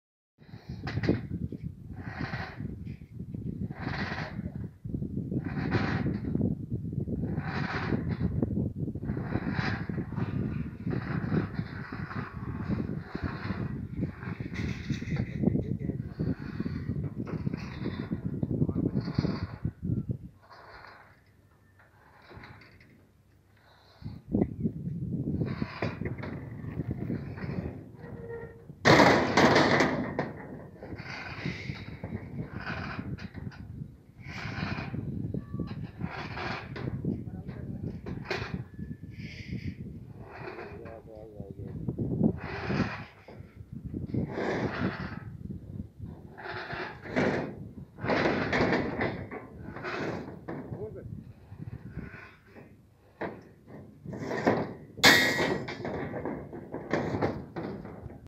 from Ground to 5th
Shifting of steel reinfircement for slab manually from Ground to 5th floor slab binding  at new Project without any tower crane or any other crane.
Contract for Labour supply
Er. Praveen Dabiwal